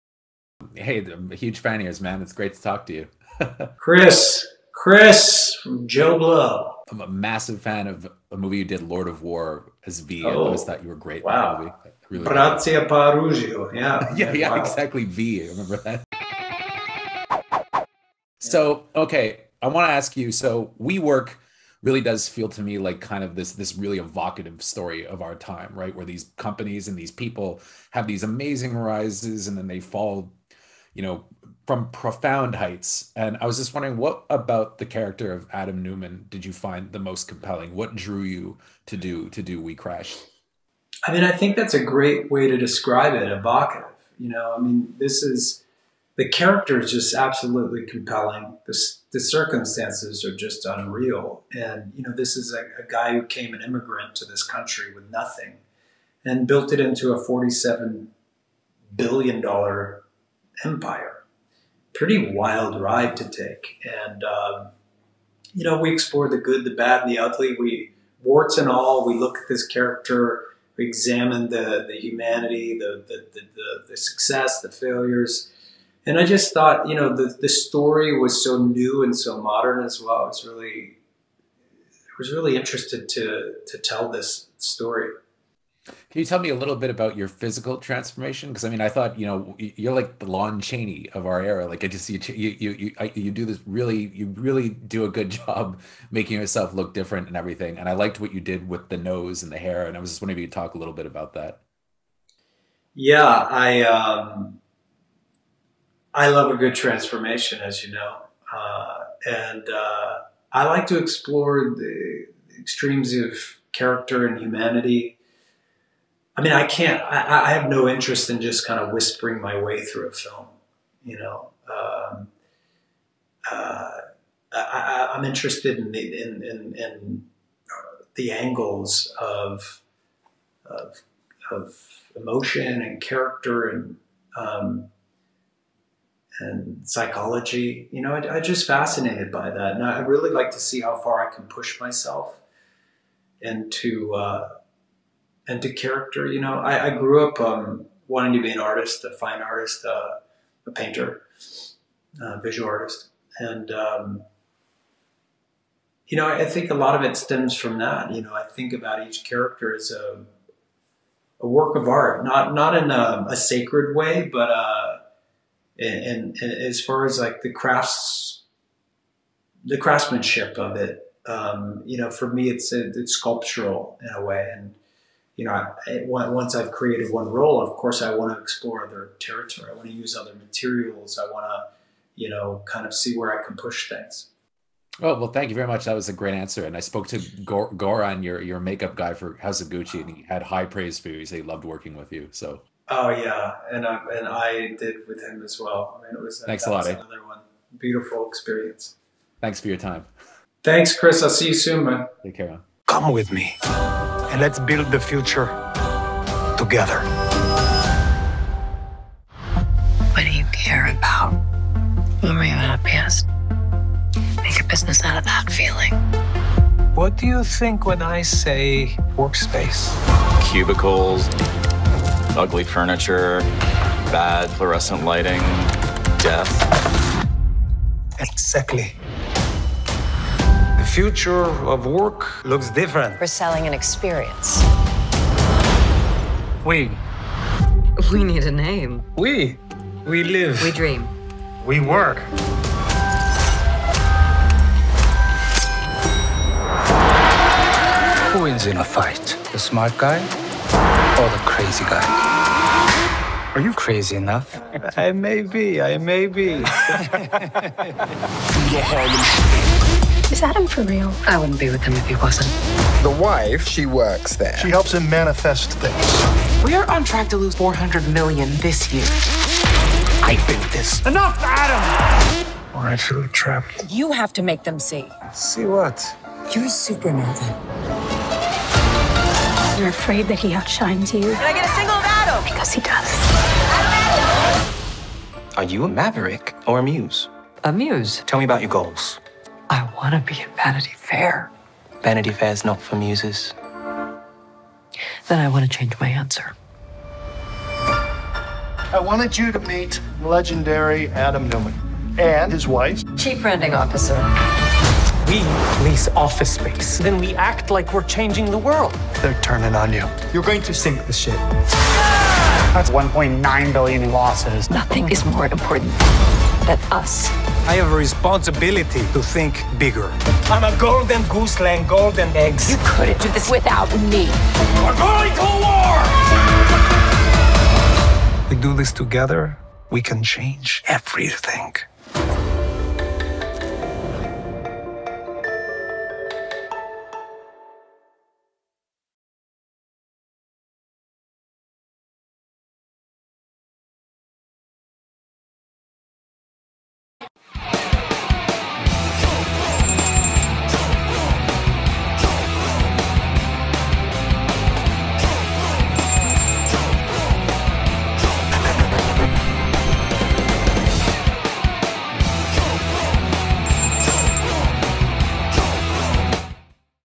Jared Leto Exclusive Interview | WECRASHED (2022) Apple TV+
JoBlo.com sits down with legendary actor Jared Leto to discuss working on his latest streaming project WeCrashed!

The rise and fall of the future of work. Stream WeCrashed March 18 on Apple TV+.

CAST: Jared Leto, Anne Hathaway